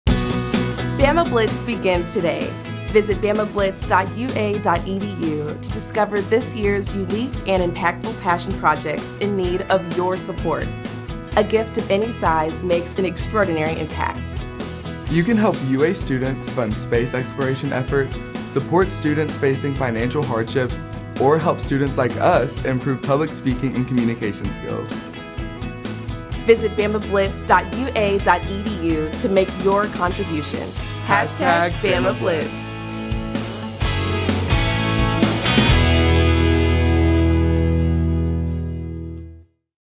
Bama Blitz Kickoff 2022 | The University of Alabama
Bama Blitz at The University of Alabama is your opportunity to bolster unique and impactful Passion Projects that would likely go unfunded without your support. Give to your favorite cause, college or program during this year's Bama Blitz, April 19-21, noon to noon.